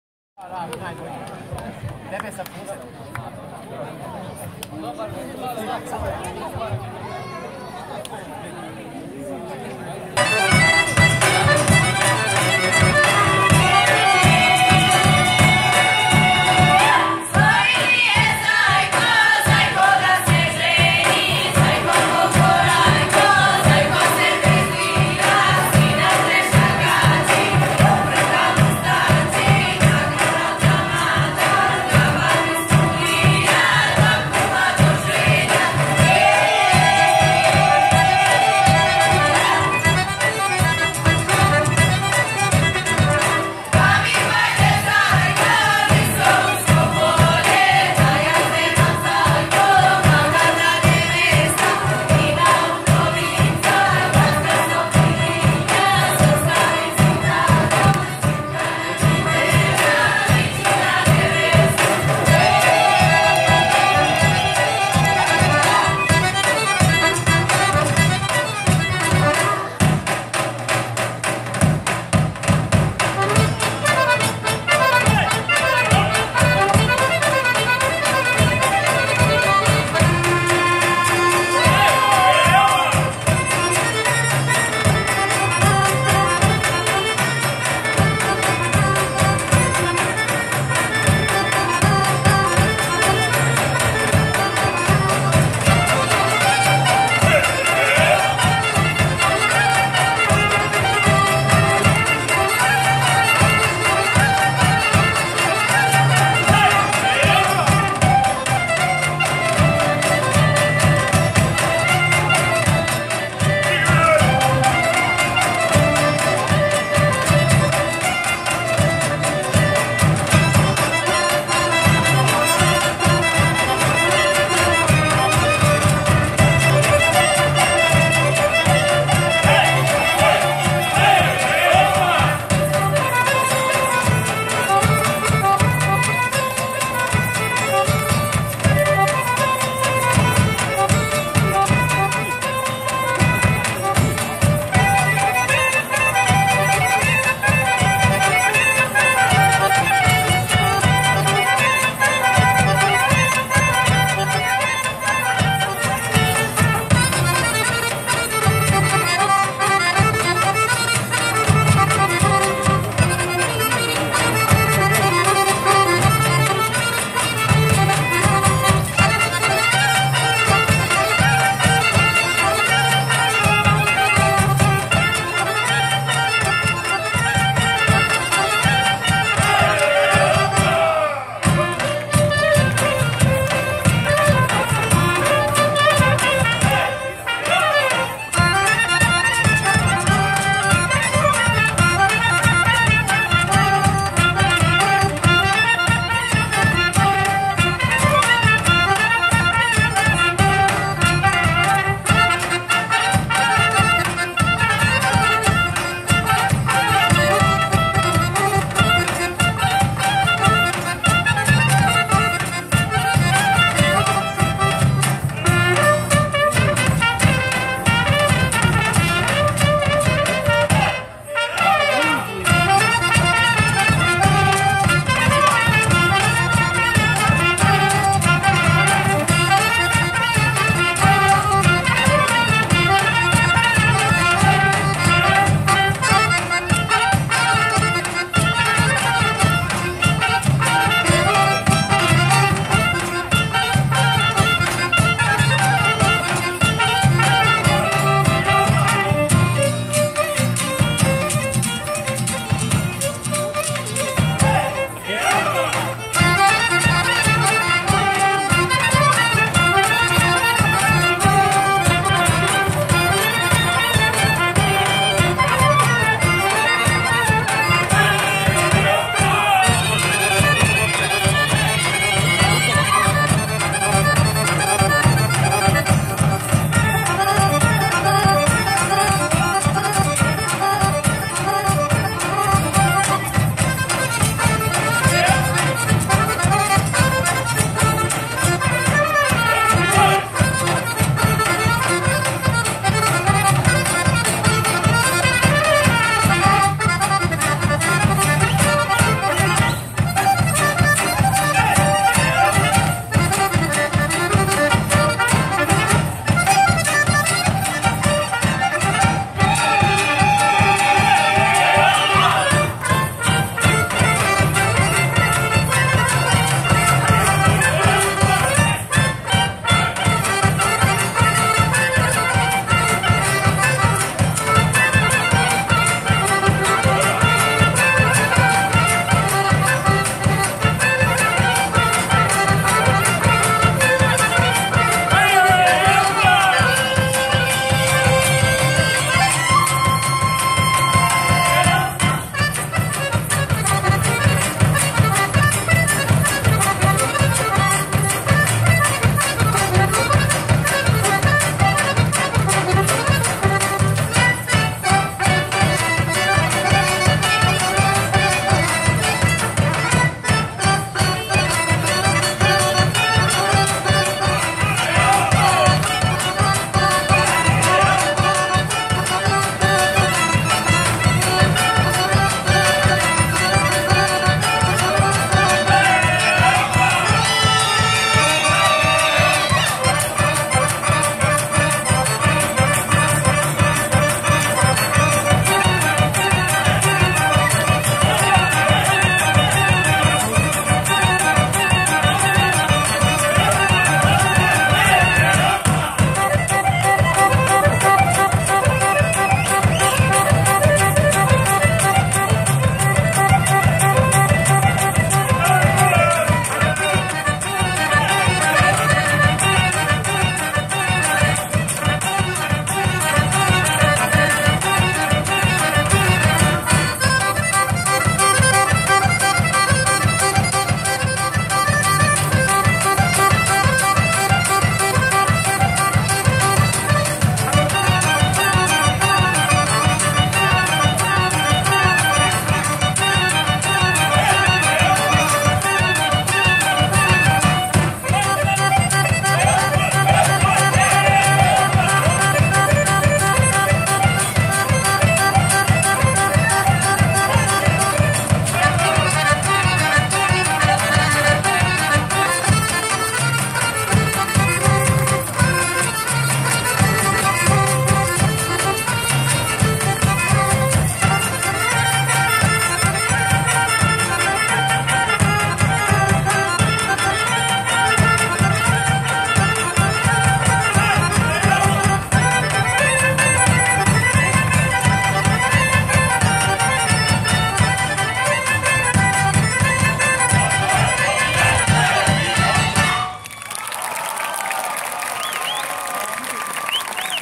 КОЗАРА ЕТНО 2️⃣0️⃣1️⃣9️⃣. КУД "Краљевачка ризница" 🇷🇸 Kozara Etno 2019. KUD "Kraljevačka riznica"🇷🇸
Бања Лука, 8.VII 2019.
Наступ КУД "Краљевачка ризница" из Краљева🇷🇸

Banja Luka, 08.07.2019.
Nastup KUD "Kraljevačka riznica" iz Kraljeva (🇷🇸Srbija)